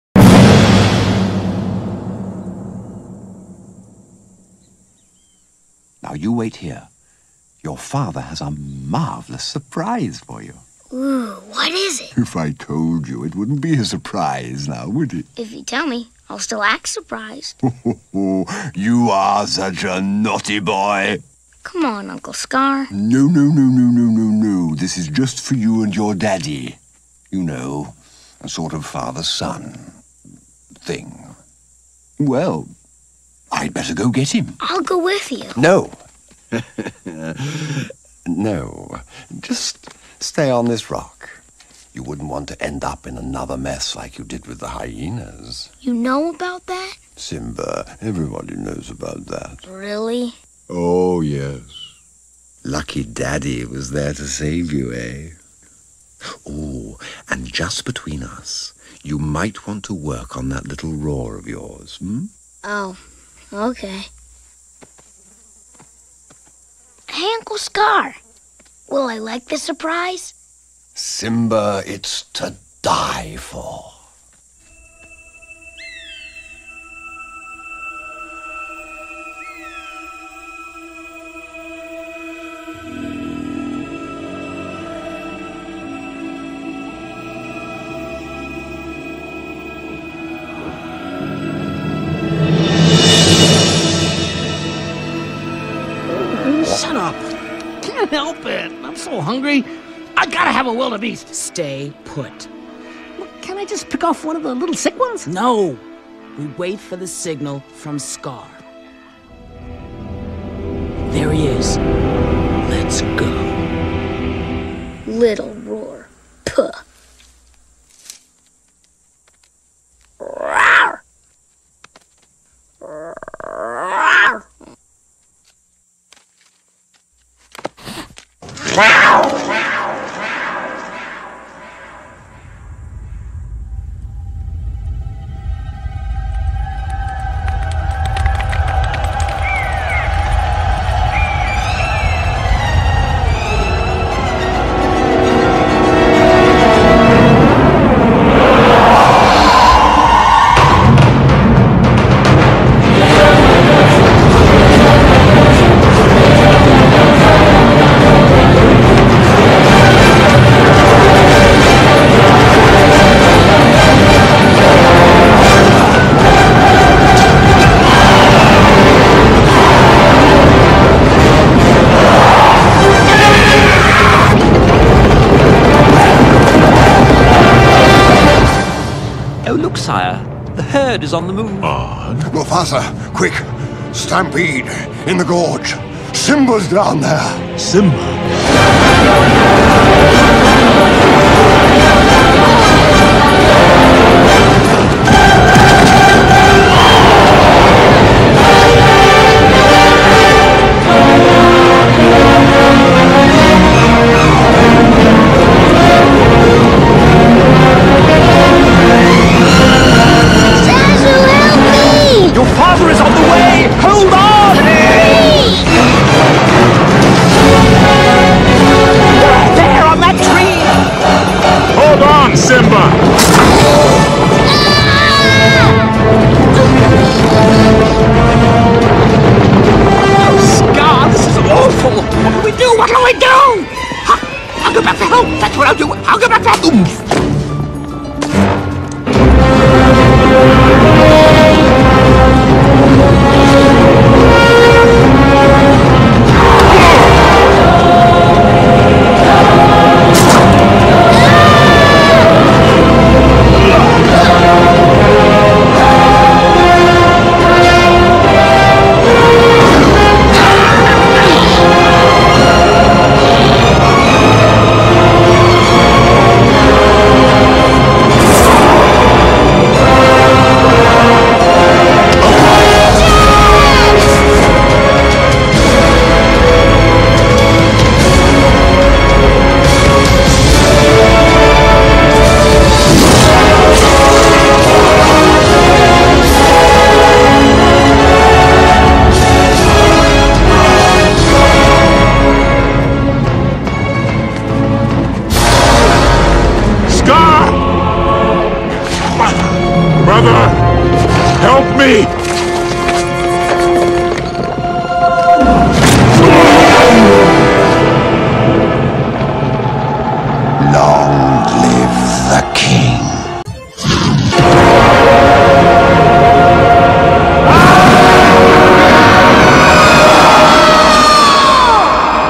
"The Plumber King " Part 9 - The Stampede
Bowser's plan start to come in to motion when Kirby gets himself in a wild runaway from a stampede.